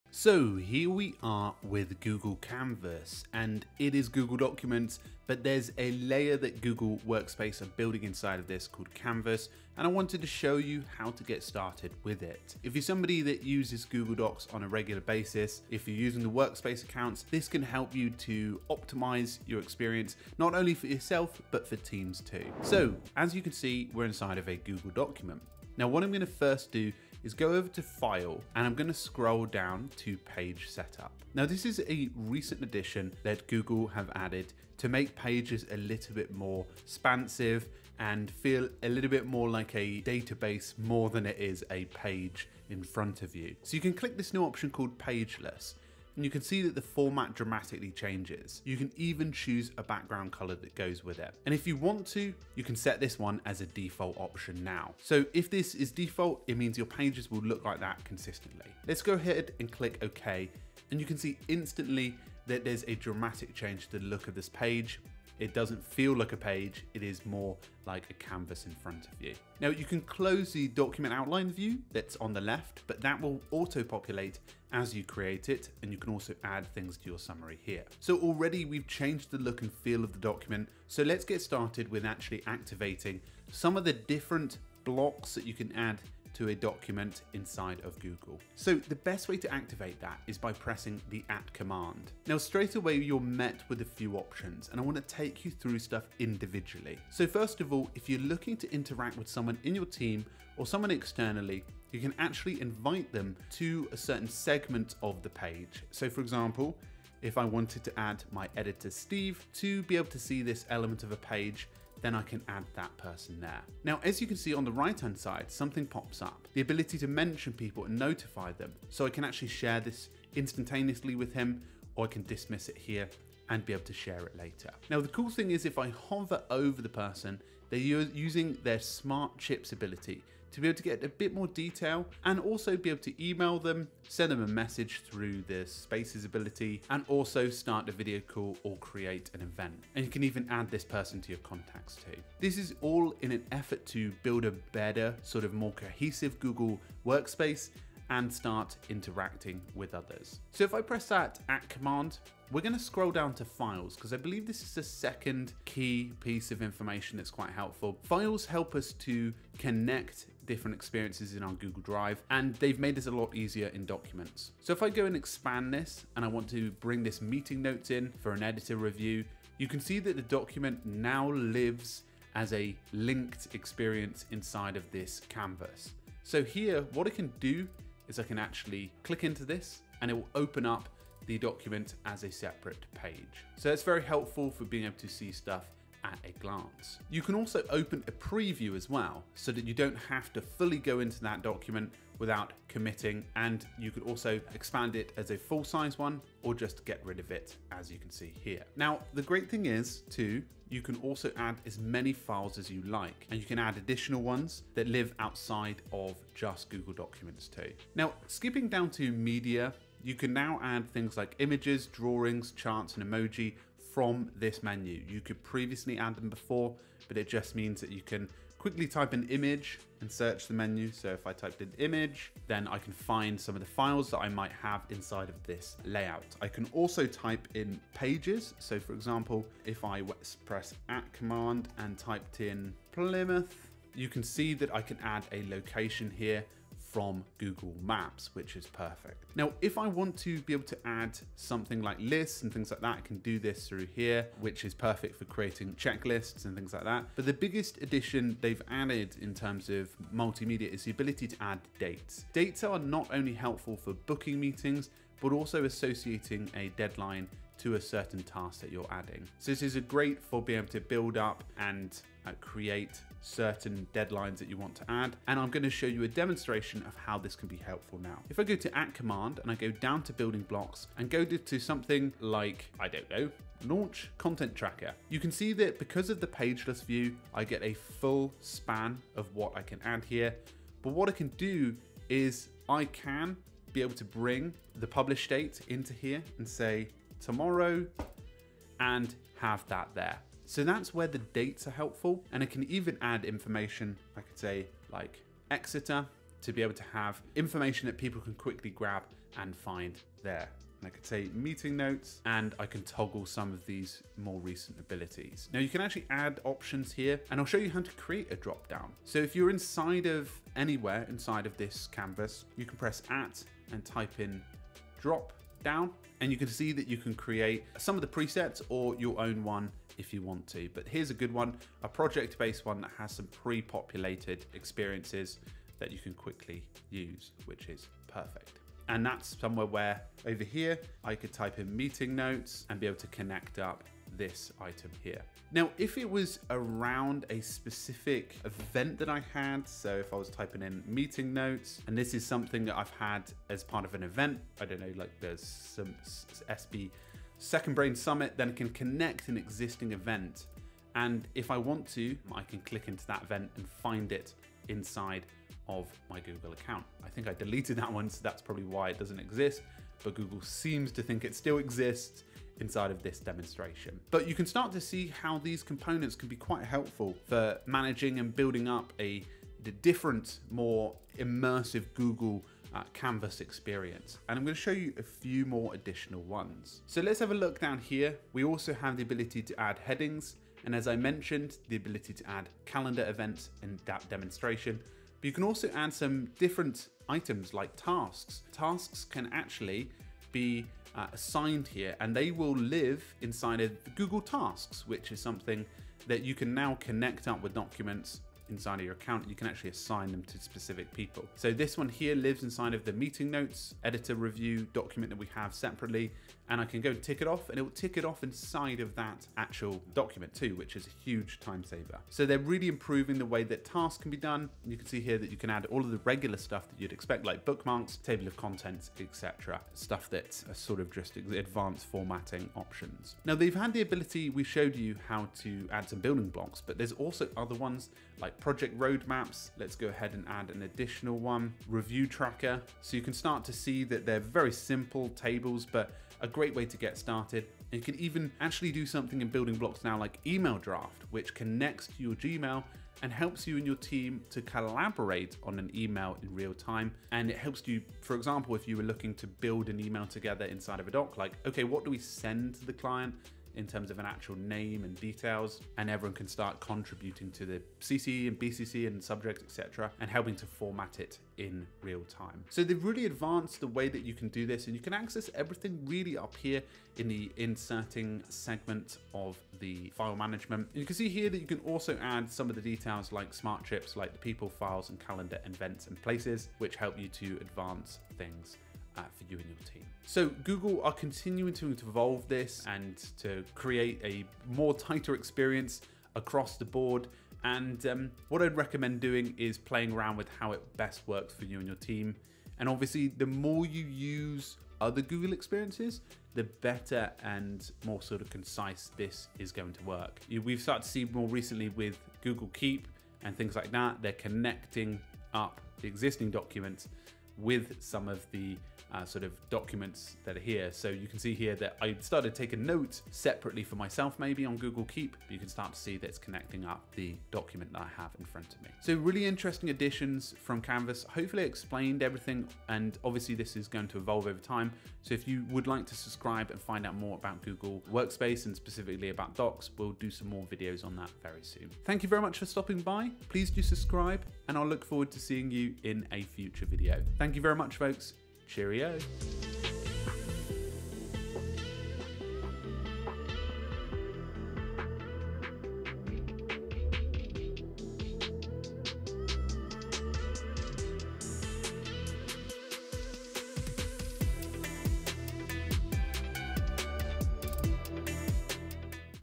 Google Smart Canvas For Dummies: Getting Started
Stop doing busywork! Try Bento Focus Find the best productivity tools with our Google Docs has been upgraded and now connects to the global Google Drive ecosystem in Google Workspace. This is how you can get started with Google Smart Canvas and improve your documents.

💻  FINAL THOUGHTS